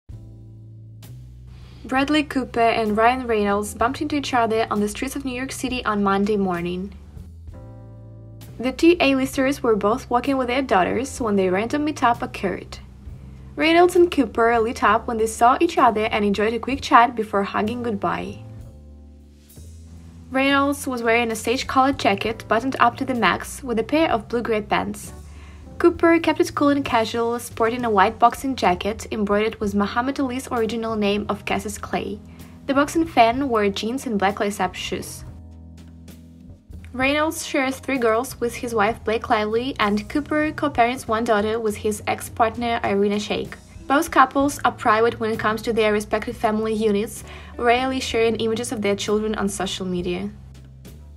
Bradley Cooper And Ryan Reynolds Randomly Bump Into Each Other On NYC Street And Share A Hug Goodbye
Bradley Cooper and Ryan Reynolds randomly bump into each other on a NYC street before having a friendly chat then sharing a hug goodbye